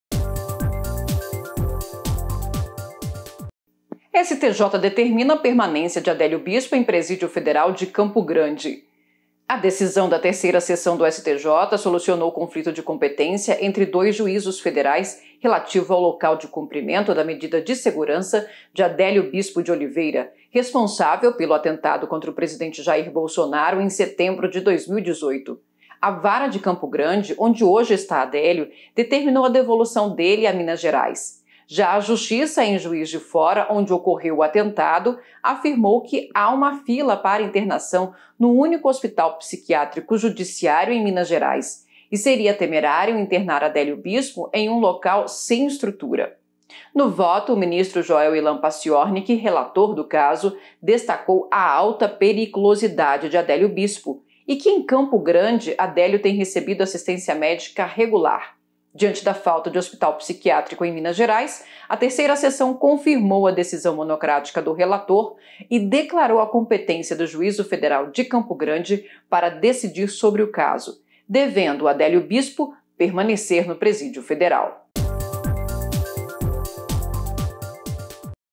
Terceira Seção determina permanência de Adélio Bispo em presídio federal de Campo Grande 17.08.20
​A Terceira Seção do Superior Tribunal de Justiça (STJ) confirmou decisão monocrática do ministro Joel Ilan Paciornik e determinou que Adélio Bispo de Oliveira – responsável pelo atentado contra o presidente Jair Bolsonaro em setembro de 2018 – permaneça na penitenciária federal de Campo Grande. A decisão solucionou conflito de competência entre dois juízos federais relativo ao local de cumprimento da medida de segurança.
CC 172812